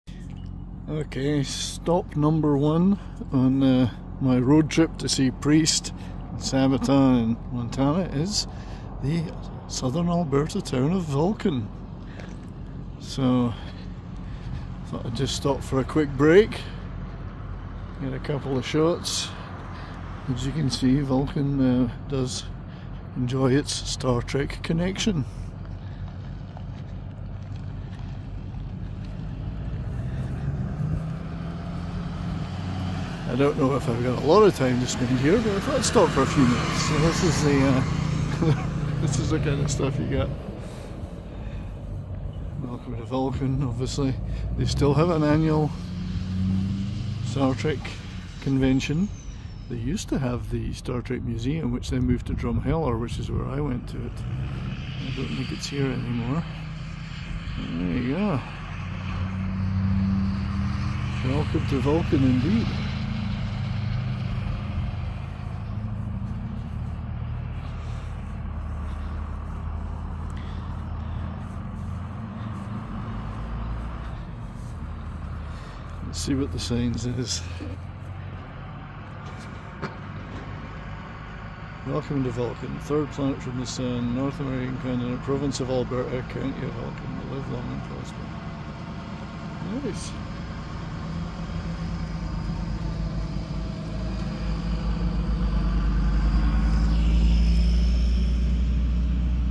Judas Priest Road Trip - Day One Stop - Vulcan, Alberta
On the road to see Judas Priest and Sabaton in Billings, Montana. On the road down, I made a quick pit stop in Vulcan, Alberta.